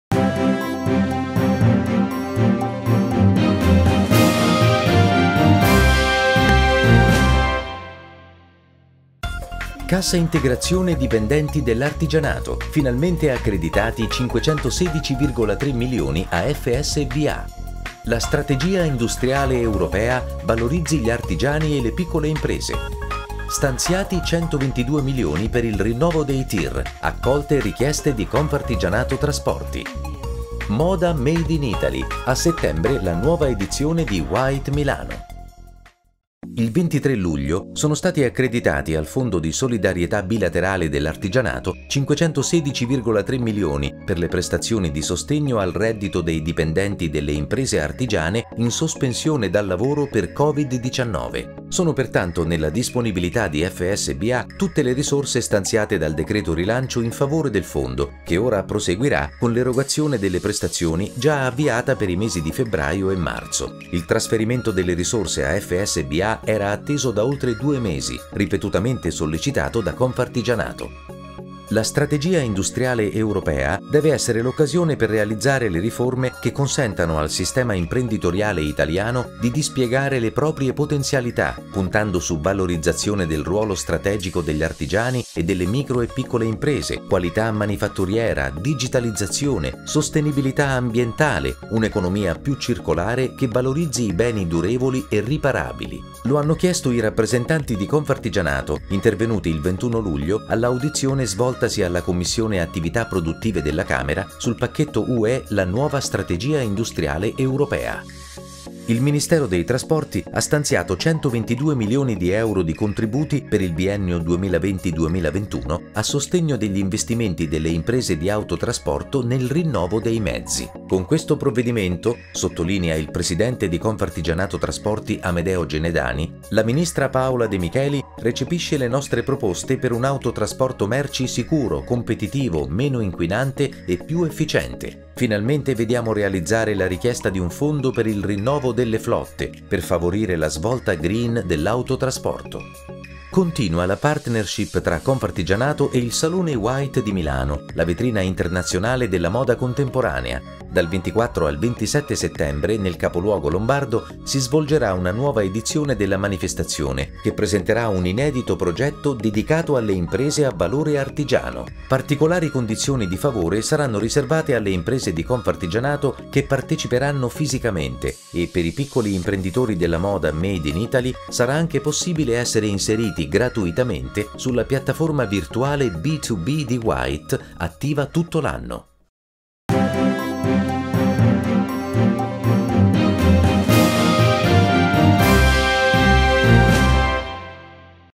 TG@ Flash Confartigianato - 24 luglio 2020
Il TG@ Flash di Confartigianato per scoprire in tre minuti tutte le notizie della settimana sul nostro Sistema, sull’artigianato e sulla micro e piccola impresa italiana. In questa edizione : dopo due mesi di attesa e le ripetute sollecitazioni di Confartigianato finalmente rifinanziata la cassa integrazione dipendenti dell’artigianato [0:36], Confartigianato alla Camera: ‘la strategia industriale UE valorizzi artigiani e piccole imprese’ [1:11], l’autotrasporto incassa 122 milioni di euro di contributi per il rinnovo dei mezzi in chiave green [1:48], a settembre moda in passerella a Milano con la nuova edizione di White [2:23].